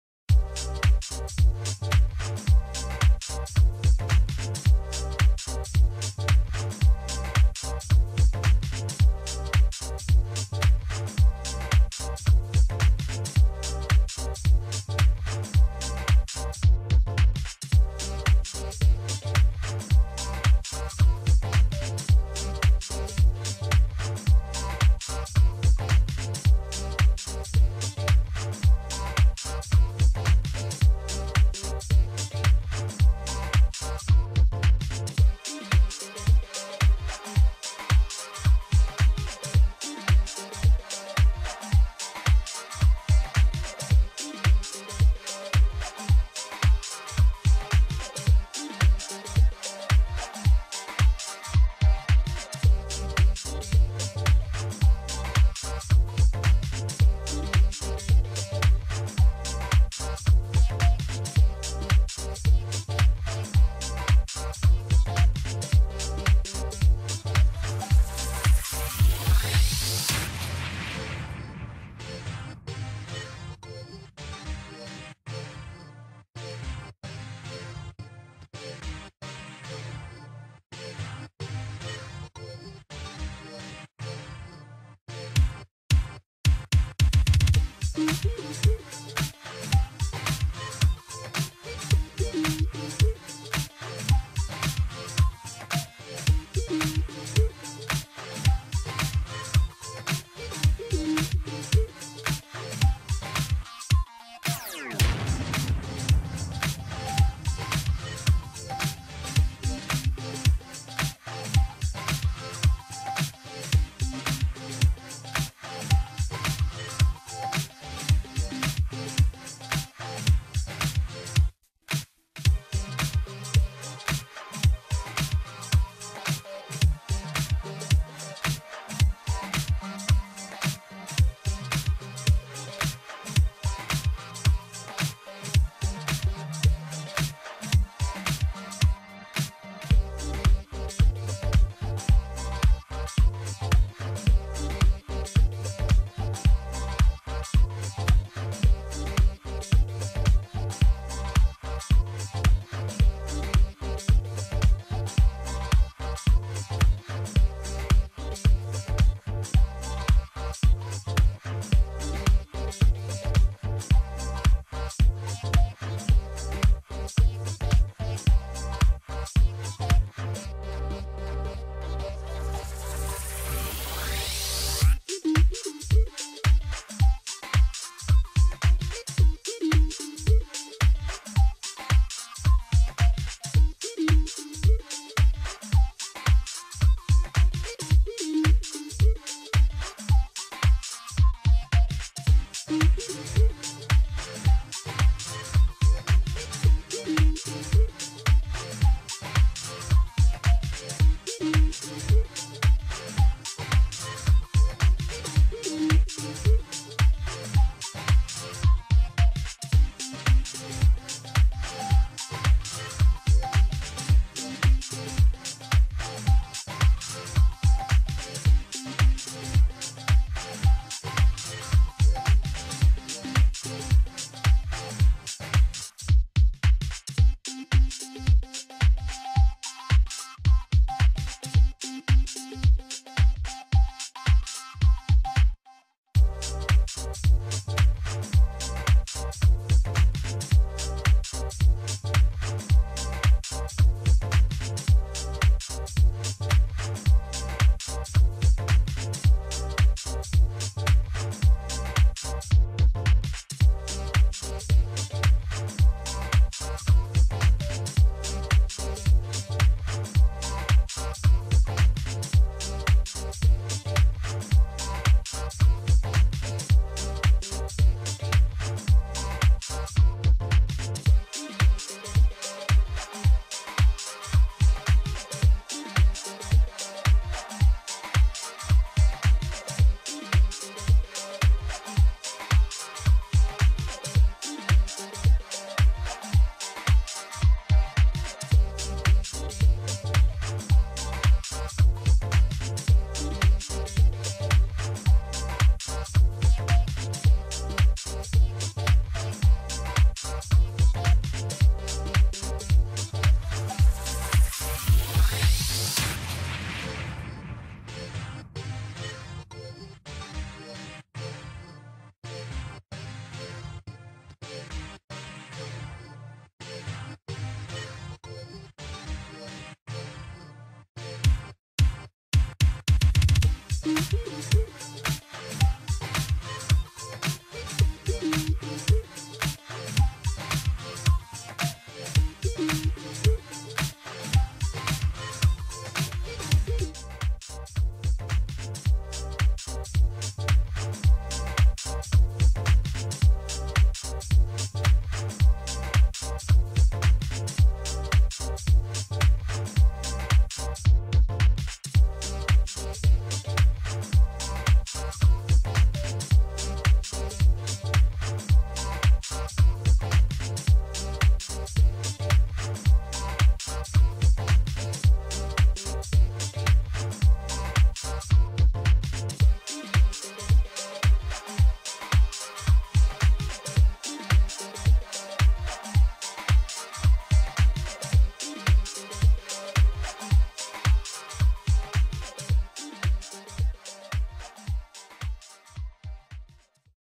Tutorial: Breastplate with Worbla's Black Art
Here I'm showing you a making of my breastplate for Angela (Spawn) by using the thermoplastic Worbla's Black Art

The music is free uncopyrighted music (informations at the end of the video)